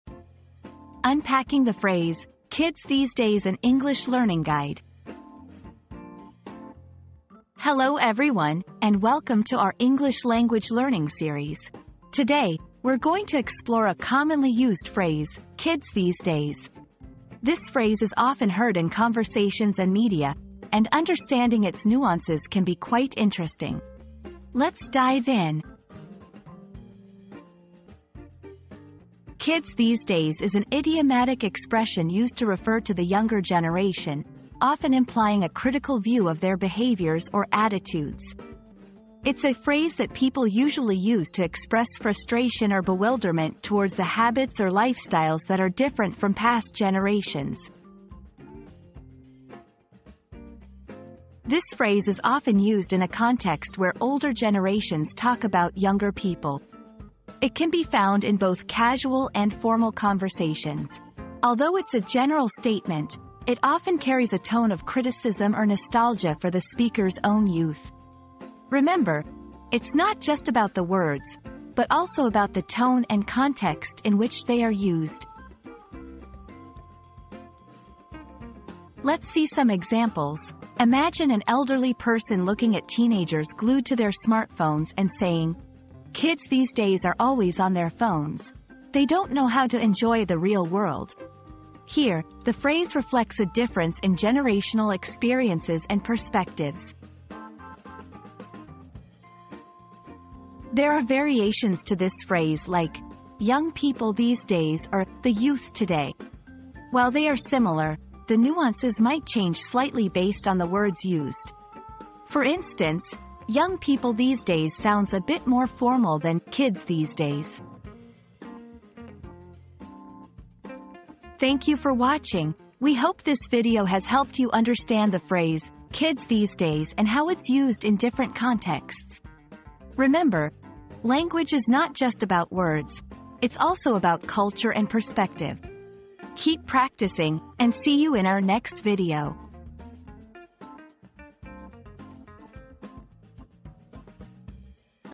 "Unpacking the Phrase: 'Kids These Days' - An English Learning Guide"
Decoding 'Kids These Days': An English Learning Guide • Join us as we unpack the meaning and usage of the popular phrase 'Kids These Days' in this informative English learning guide. Discover the nuances and cultural context behind this expression and enhance your language skills with practical examples and insights.

00:00 • Introduction - "Unpacking the Phrase: 'Kids These Days' - An English Learning Guide"
00:28 • Understanding the Phrase
00:50 • Context and Usage
01:18 • Examples in Sentences
01:40 • Variations of the Phrase